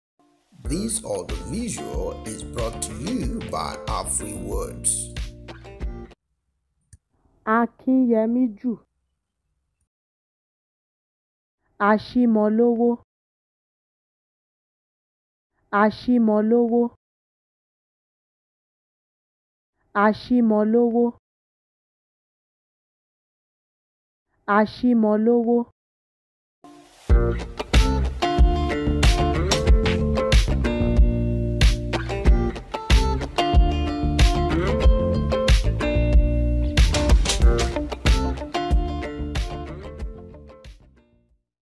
How To Pronounce AKINYEMIJU
Meaning of Akínyẹmíjù

Valor suits me the most.